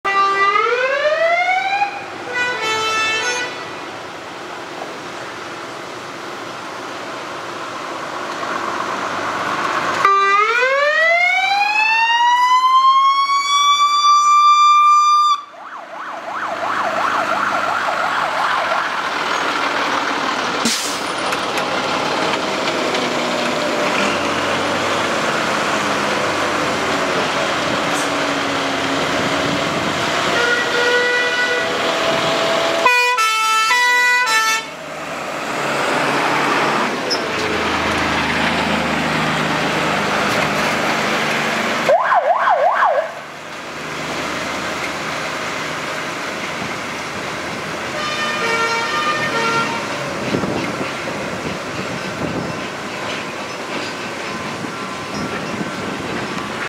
Bressingham Fire Engine Rally - Ex Buckinghamshire 1994 Renault Appliance + Bedfordshire Volvo FL6
Thanks to these 2 guys that owned these old fire appliances for making some noise for us on the way out really appreciated it.